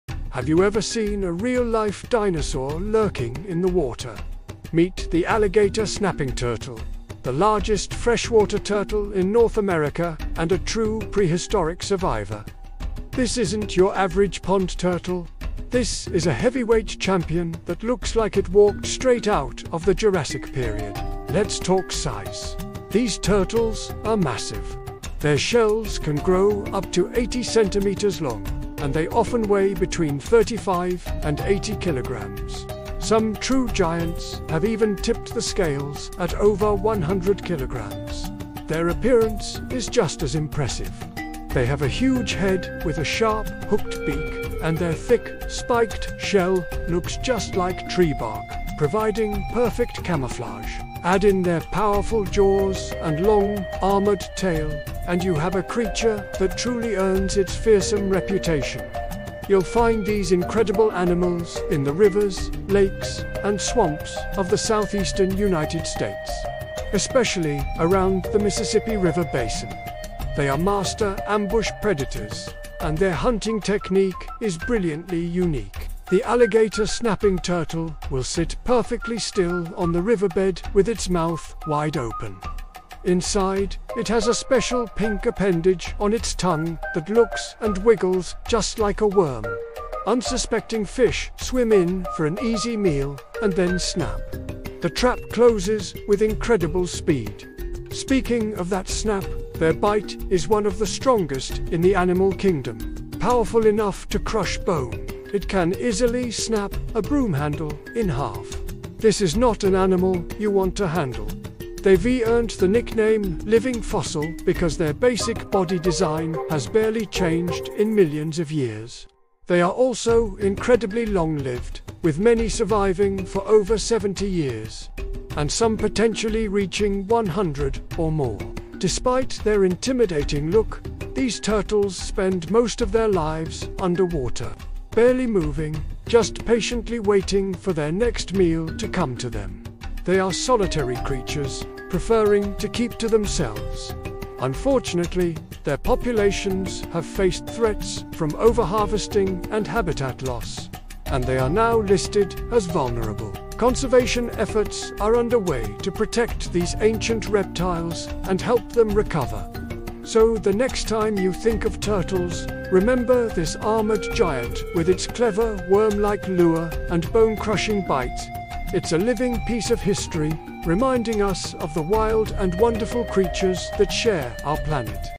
Alligator Snapping Turtle fypシ゚
Alligator Snapping Turtle (Macrochelys temminckii)

The largest freshwater turtle in North America, famous for its dinosaur-like look and bone-crushing bite.

Key Facts

Size:
Shell length up to 80 cm (31 in)
Weight commonly 35–80 kg, with rare giants over 100 kg

Appearance:

Massive head with hooked beak

Thick, spiked shell that looks like tree bark

Powerful jaws and long tail

Rough, armored skin

Where It Lives:
Southeastern United States — rivers, lakes, swamps, and bayous, especially the Mississippi River basin.

Diet:
Carnivorous — fish, frogs, snakes, birds, small mammals, carrion.

What Makes It Unique

➤ Worm-shaped tongue lure
It sits motionless with its mouth open and wiggles a pink, worm-like appendage on its tongue. Fish swim in… and SNAP.

➤ One of the strongest bites in the animal kingdom
Its jaws can crush bones, easily snapping broom handles and turtle shells.

➤ Living fossil look
Its body design hasn’t changed much for millions of years, earning it a prehistoric reputation.

➤ Extremely long lifespan
Can live 70–100+ years.

Behavior

Mostly aquatic; rarely leaves water

Ambush predator — barely moves

Aggressive when handled on land

Slow swimmer but lightning-fast strike

Solitary except during mating

Conservation Status

Vulnerable (and protected in many states).
Threats include:

Overharvesting in the past

Habitat destruction

Bycatch and pollution

Conservation efforts are helping populations recover.

Fun Fact

Despite their terrifying look, alligator snapping turtles spend most of their lives completely still — waiting patiently for prey to come to them.